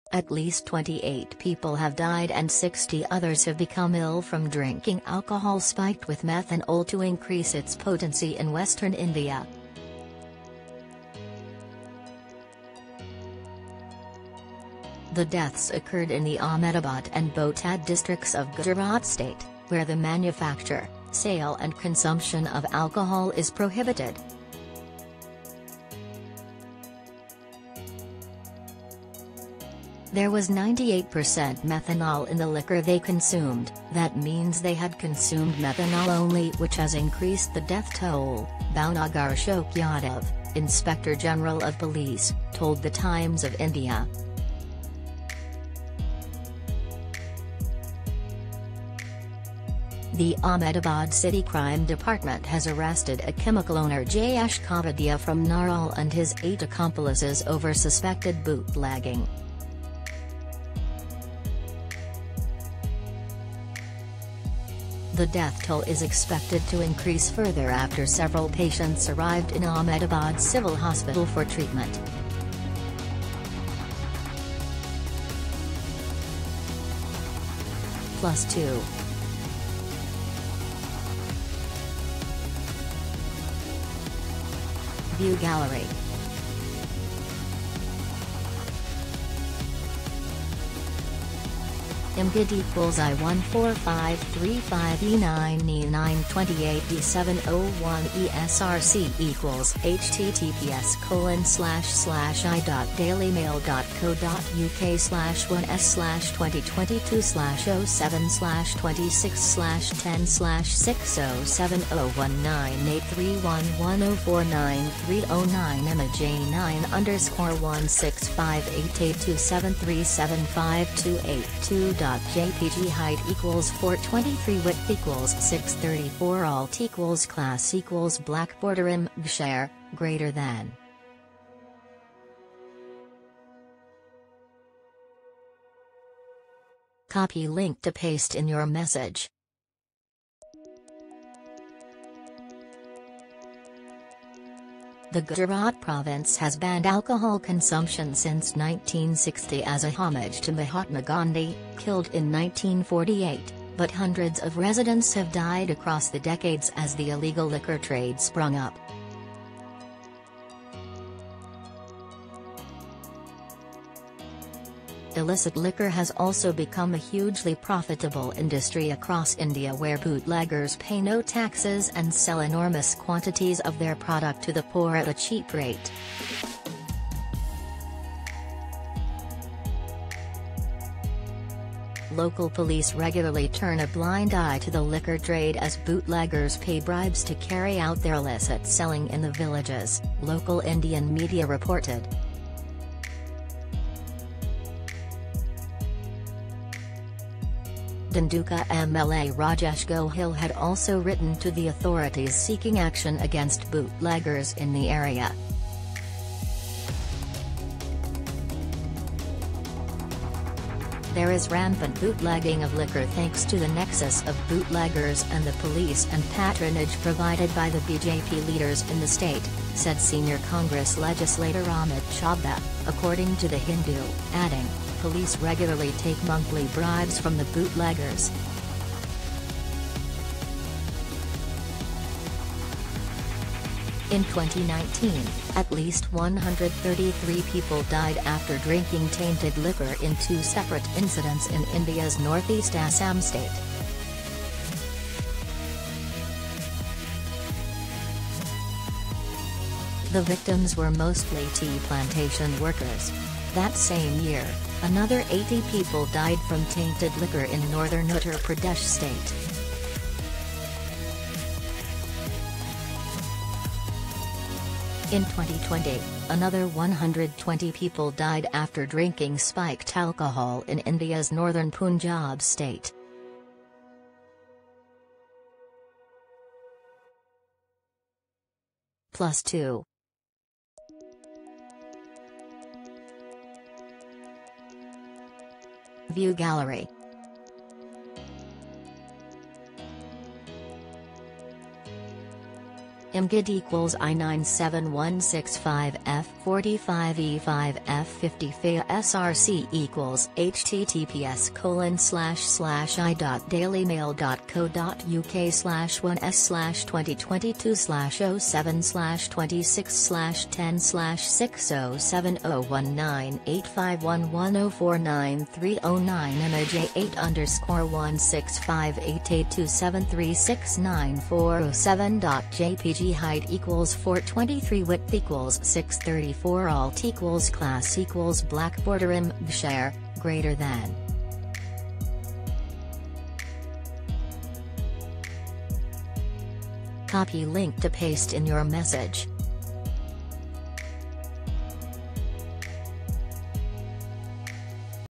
28 people die and 60 others are ill after drinking bootlegged alcohol spiked with methanol in India
At least 28 people have died and 60 others have become ill from drinking alcohol spiked with methanol to increase its potency in western India.
The deaths occurred in the Ahmedabad and Botad districts of Gujarat state, where the manufacture, sale and consumption of alcohol is prohibited.
'There was 98 percent methanol in the liquor they consumed, that means they had consumed methanol only which has increased the death toll,' Bhavnagar Ashok Yadav, inspector general of police, told The Times